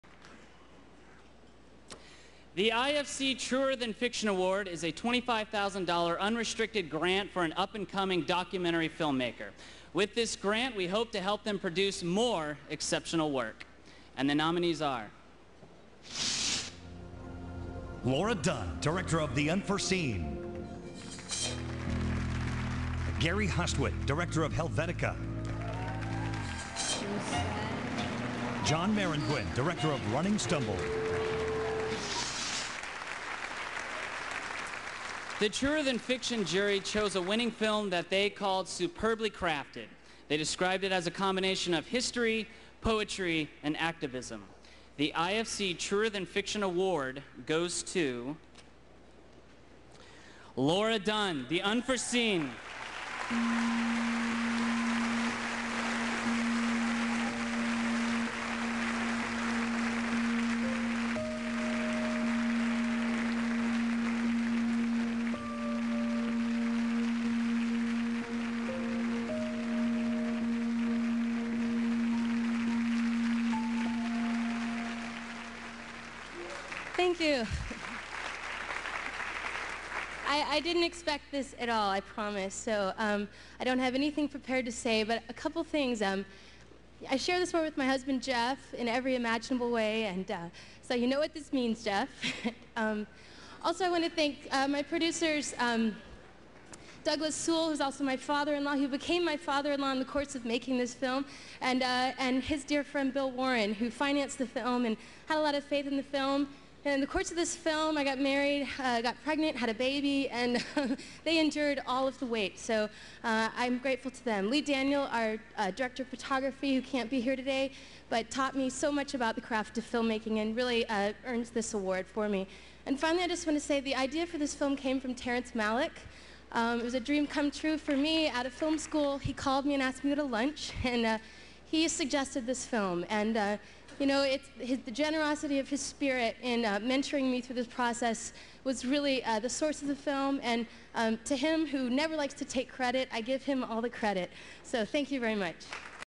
Laura Dunn receives Spirit Award for "The Unforeseen"
Director Laura Dunn accepts the IFC "Truer than Fiction" Independent Spirit Award for her debut documentary "The Unforeseen."

Executive produced by Terrence Malick and Robert Redford, "The Unforeseen" has been called "The best film of [2007 Sundance Film] festival, hands down." by Gavin Smith of Film Comment.

Released by Two Birds Film, Despair.com, Cinema Guild and The Sundance Channel.